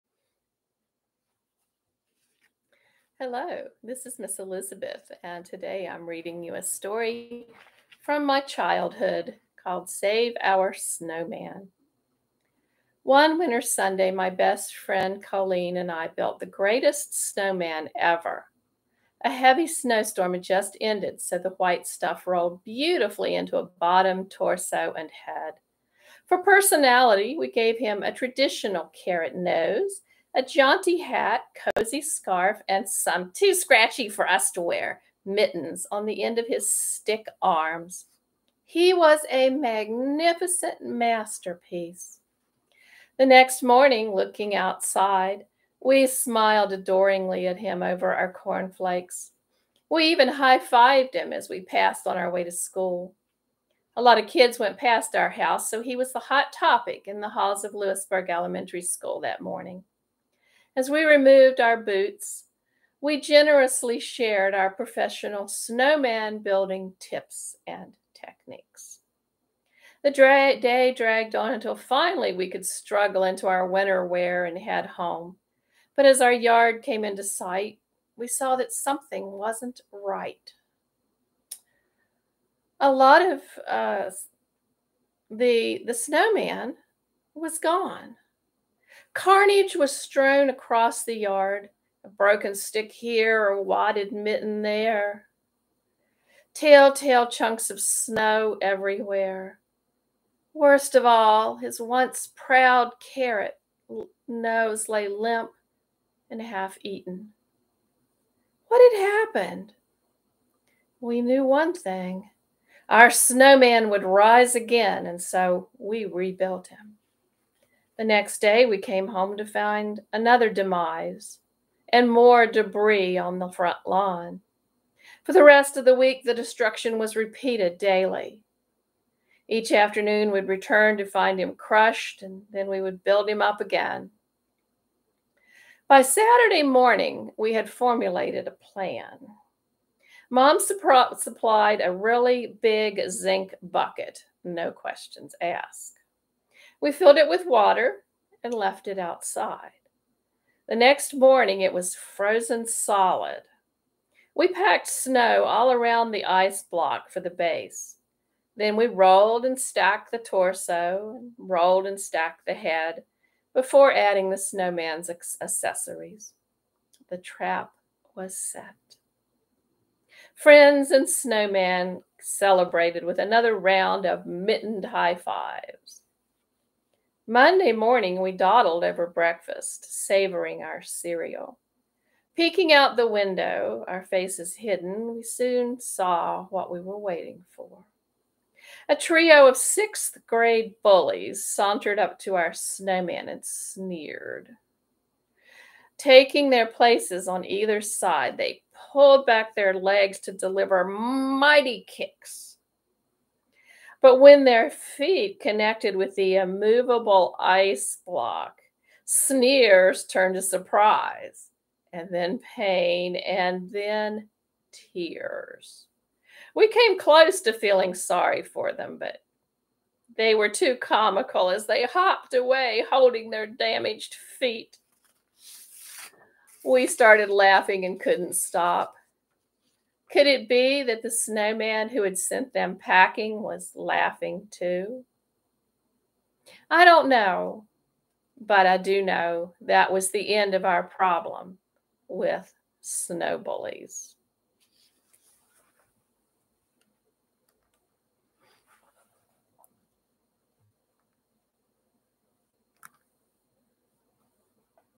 Save Our Snowman
A winter short story by Elizabeth Spangler
Appropriate for grades 3 and up.